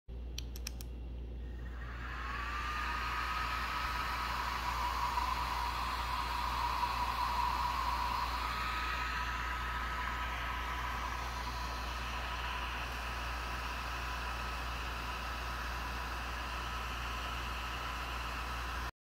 Imalent MS18 broken right out of box.
Brand new Imalent MS18 with only one fan operational. The fan that is not working is not stuck. It spins freely. Because only one fan is working, the light steps down from turbo mode in approximately 15 seconds, with the head getting hot enough to burn skin. This should have been tested before shipping to customers.